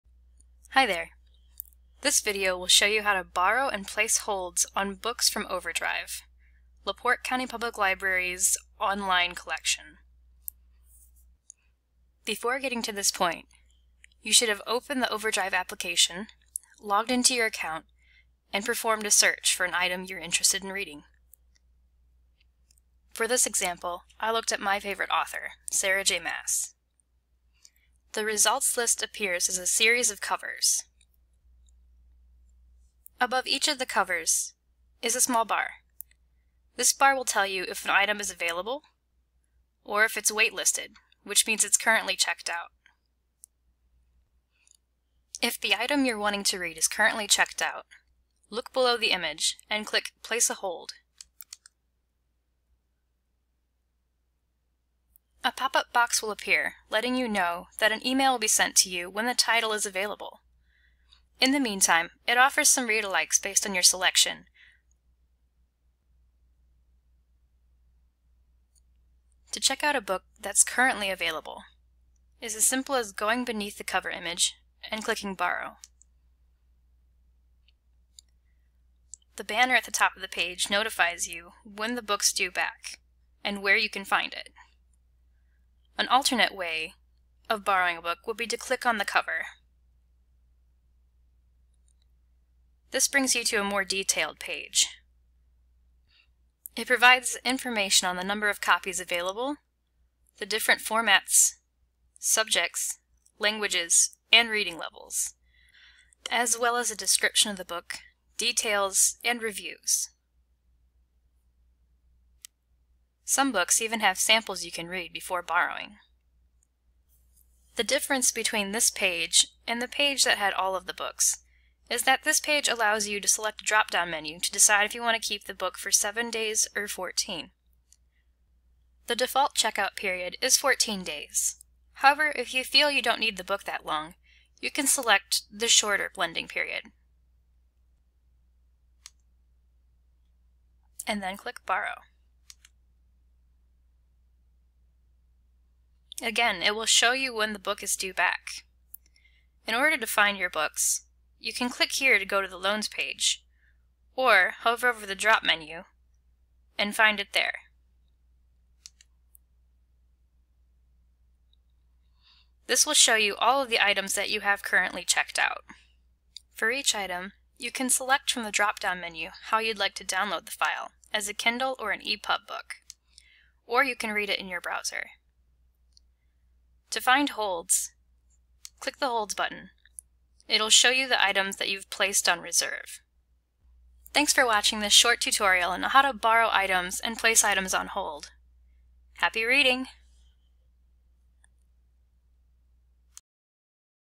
Borrowing and Placing Holds on Overdrive (La Porte County Public Library)
This short video shows you how to borrow or reserve digital items, such as e-books and e-audiobooks, through Overdrive Digital Media at La Porte County Public Library.